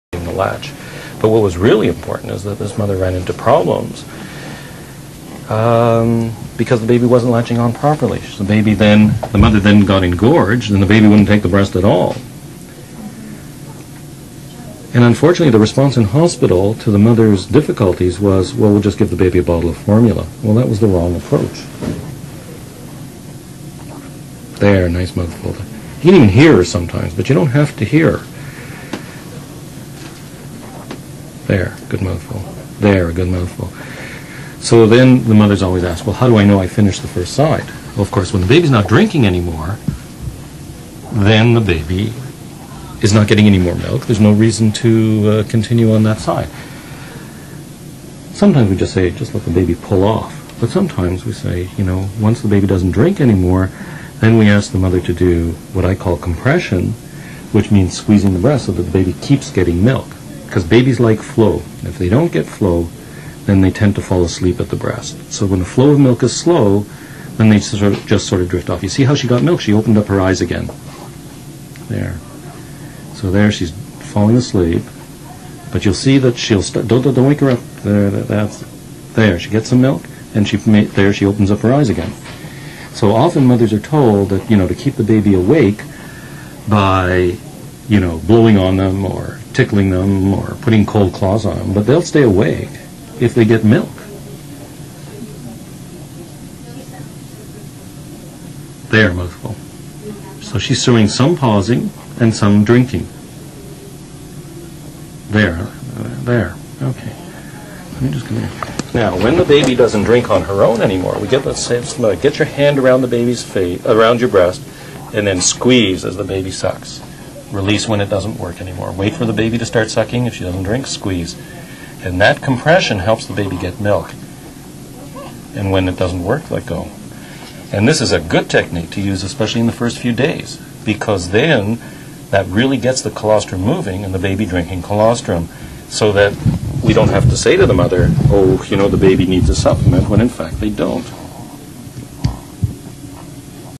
Dr Jack Newman Breastfeeding Video - Compression when baby falls asleep
Young babies tend to fall asleep at the breast when the flow of milk slows.  This clip shows that as the baby gets more milk, she opens her eyes.  The technique of compression is shown.